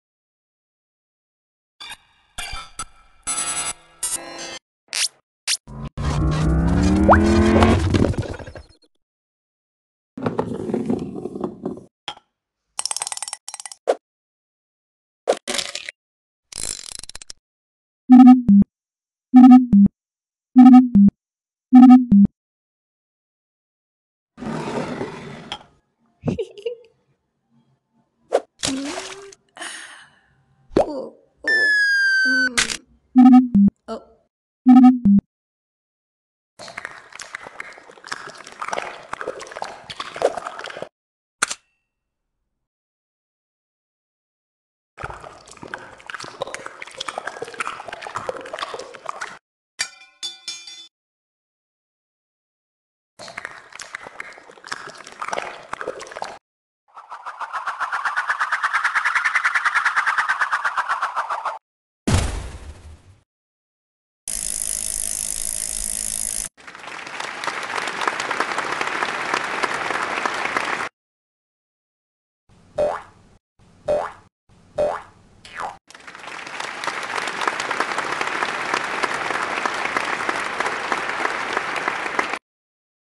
My YOUTUBE logo Animation Compilation | STOP MOTION
This is a compilation of all my claymation "Shorts" I made so far, with Youtube as the Protagonist.
Each short is separately uploaded on my channel as "Shorts".